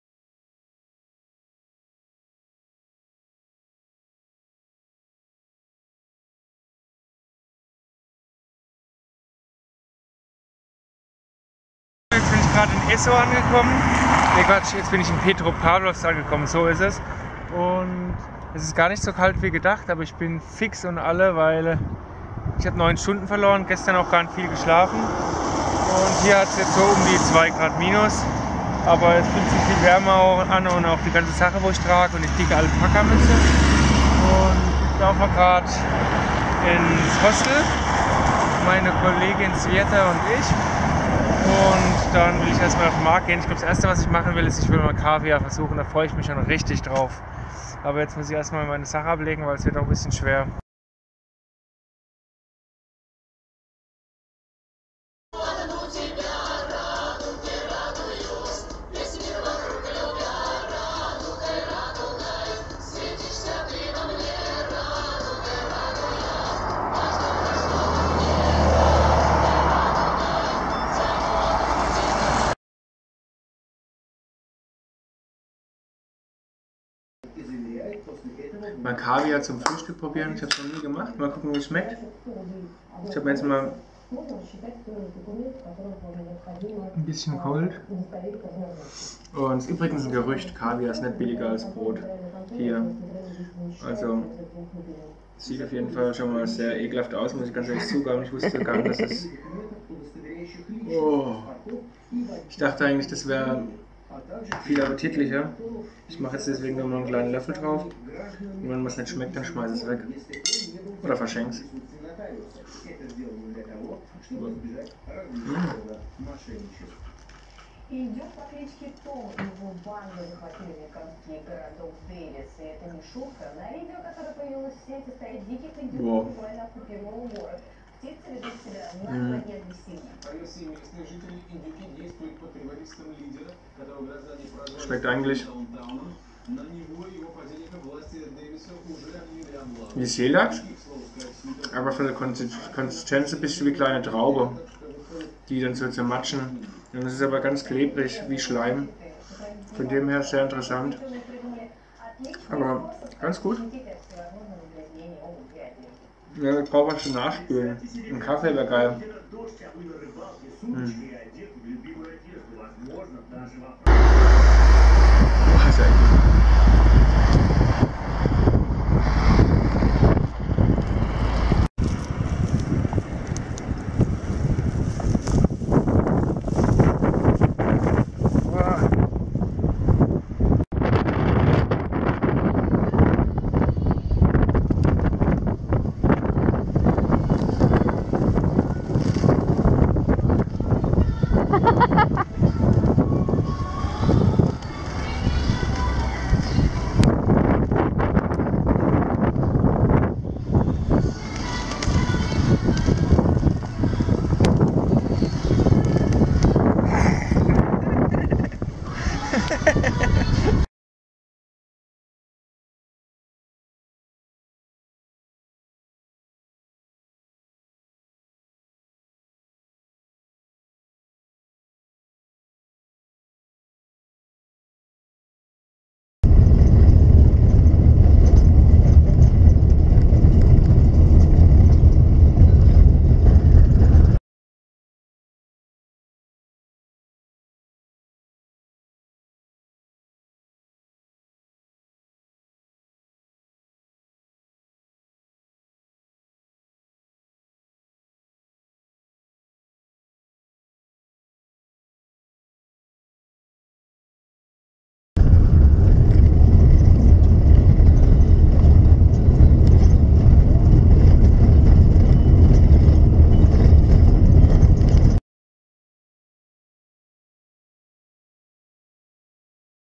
#003 Reise nach Kamtschatka (Russland) Teil 3
Travelblog: Geschafft! Heute lande ich in Elizowo (Елизово), einer kleinen Stadt in der Nähe von Petropawlowsk (Kamtschatka)!
Bei der Ankunft hatte es milde -14 Grad und ich war ziemlich müde von den 9 Stunden Zeitunterschied zu Moskau.

Der Flug mit Aeroflot war super, auch wenn ich nicht viel geschlafen habe.

Zwei Nächte blieb ich im Hostel24 ...

Zudem sind einige Sequenzen bei der Anreise mit Kameras gemacht, weshalb die Qualität nicht optimal ist, ... im Zweifelsfall kann jeder selbst die Bildqualität unten am Rädchen verändern ;)